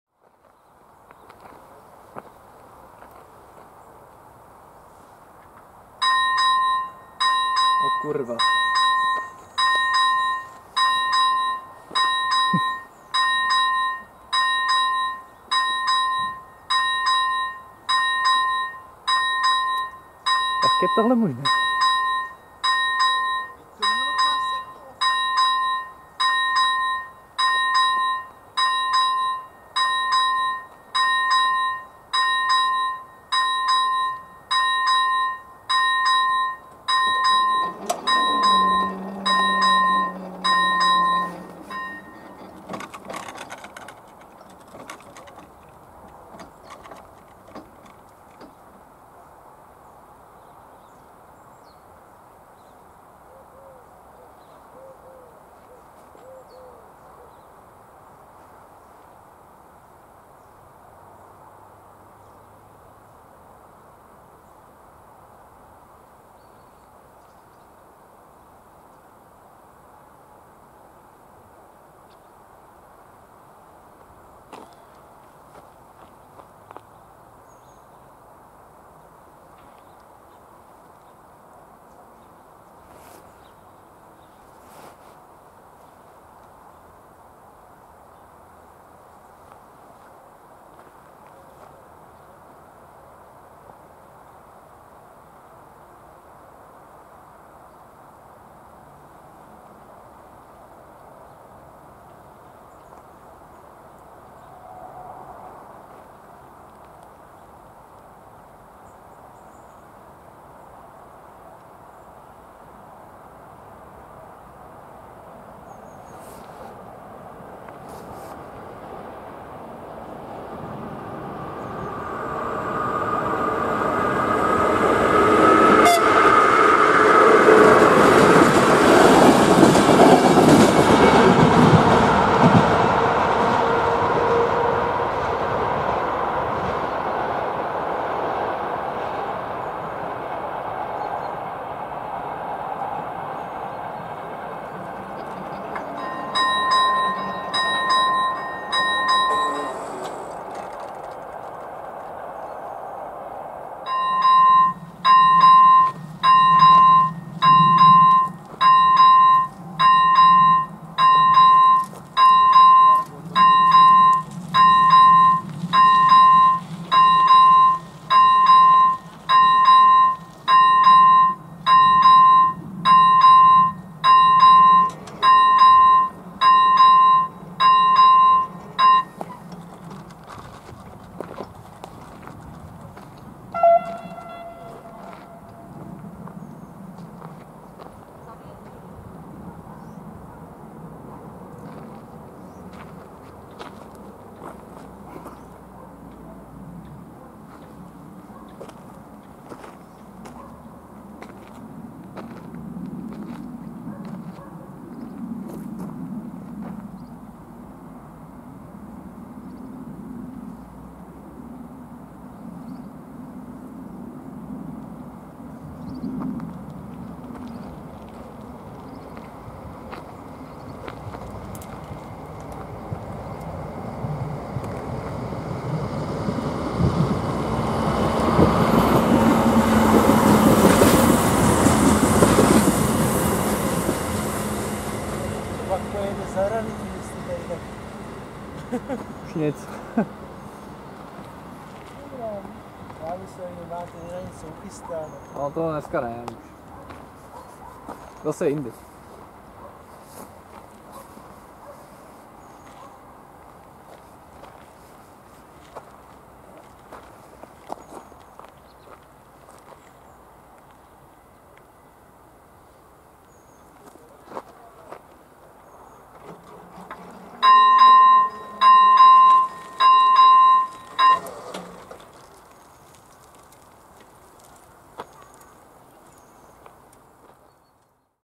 Železniční přejezd Týniště nad Orlicí #1 [P4023] - 23.9.2016 / Czech railroad crossing
Lokalita / Location: Týniště nad Orlicí, ul. Nádražní
Trať / Track: SŽDC 020 (Velký Osek - Choceň), 026 (Týniště nad Orlicí - Broumov)
Úsek / Section: Týniště nad Orlicí - Petrovice nad Orlicí / Bolehošť
Výstražníky / Lights: AŽD 71 + elektronický zvonek 1. generace
Vlaky / Trains:
1) Os 5206 (Týniště nad Orlicí - Chlumec nad Cidlinou)
2) Os 5133 (Hronov - Doudleby nad Orlicí)
Děkuji za super den uživateli Michal Veselka.